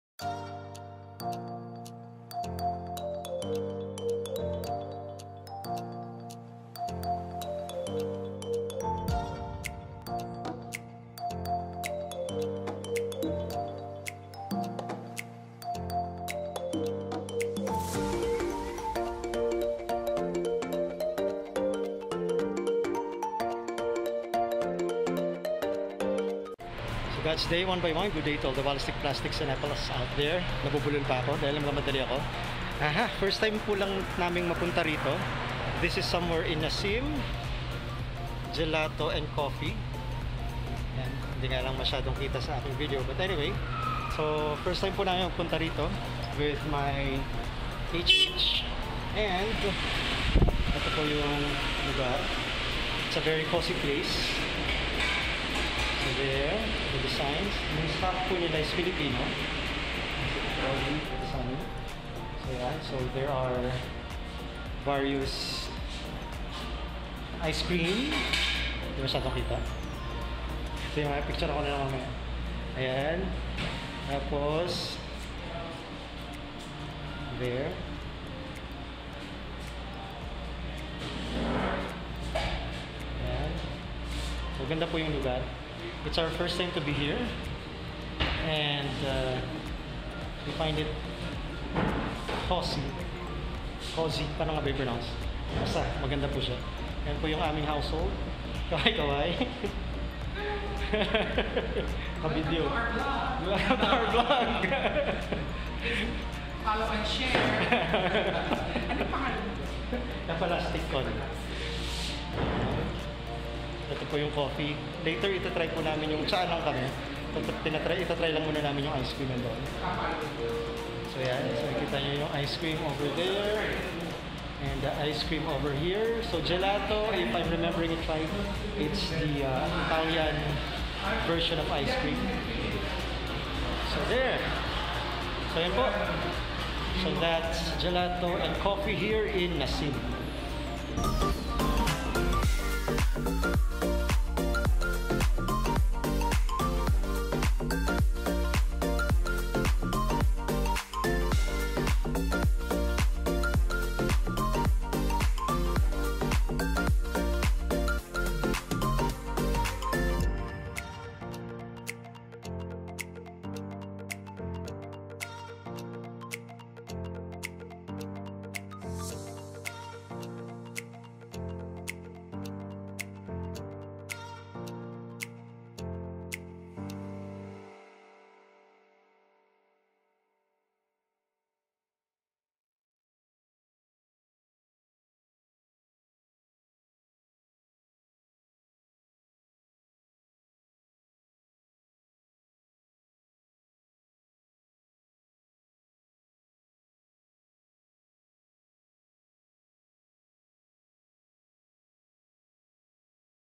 Jelato and Coffee Experience / Vid166 / 2022 / Epalastic Con
Dahil FirstTimer kami sa JelatoandCoffee sa Nasseem, for our LowerHHBonding, ay naisipan kong i-vlog ng mabilisan ang experience, just to introduce this newest discovery.